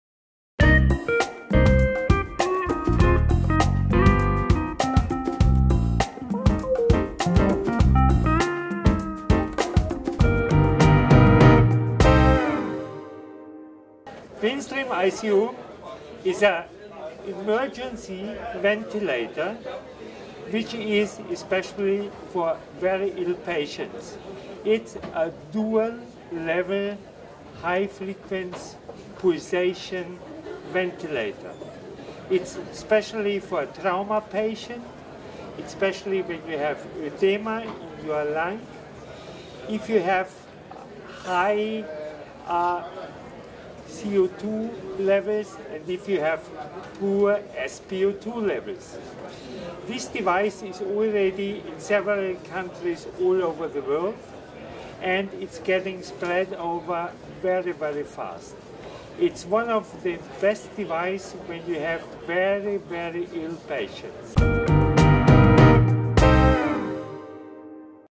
What is TwinStream ICU? Herbert Murlasits
I Expert: Herbert Murlasits, Director International Sales, CARL REINER, Vienna, Austria
I Question: What is TwinStream ICU?
I Minute: Just to the point!

TwinStream ICU is an emergency ventilator which is especially for very ill patients. It is a duel level high frequency position ventilator. It is especially for a trauma patient, especially when you have a tumor in your lung. If you have high CO2 levels and poor SpO2 levels. This device is already in several countries all over the world and it is getting spread over very, very fast. It is one of the best device when you have very, very ill patients.